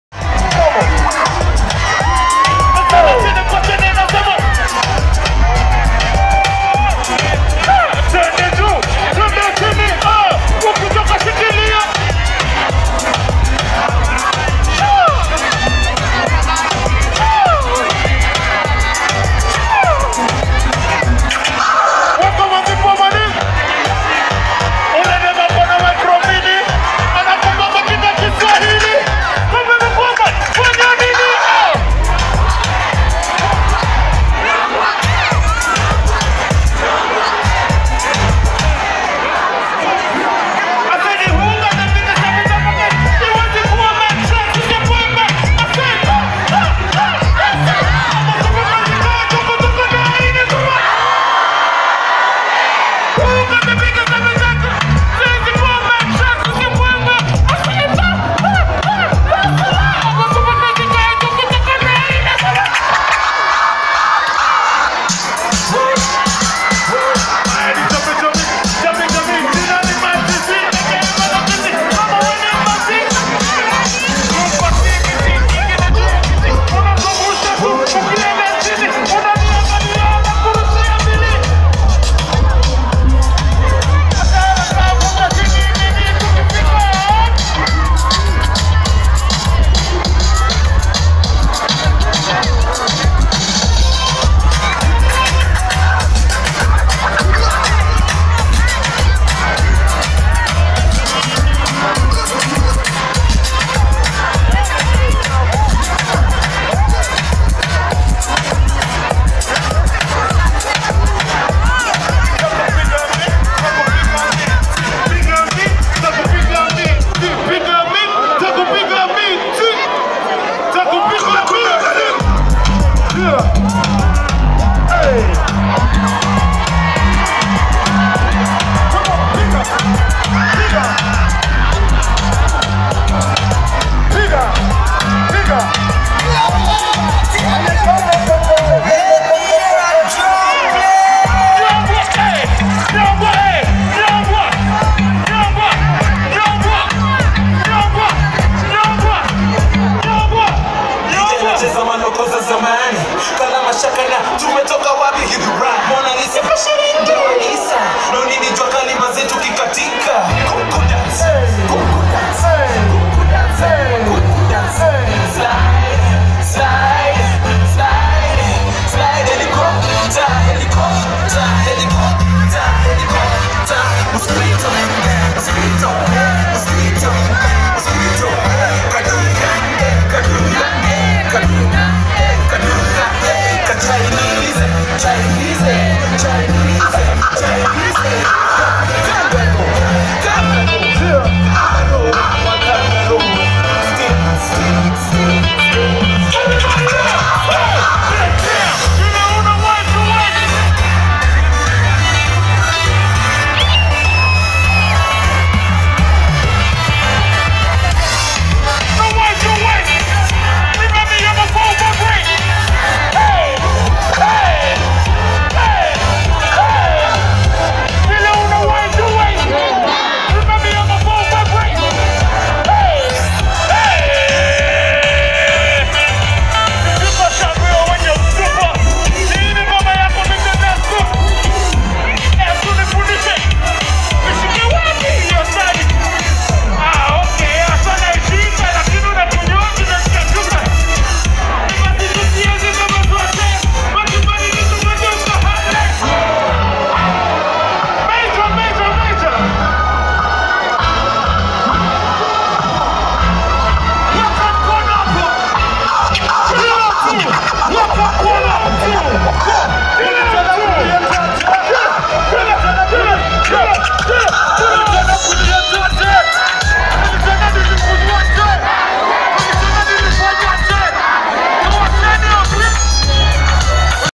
THE KANSOUL PERFORMING AT THE WAVE explicit
THE KANSOUL PERFORMING AT THE WAVE explicit  at tekon concert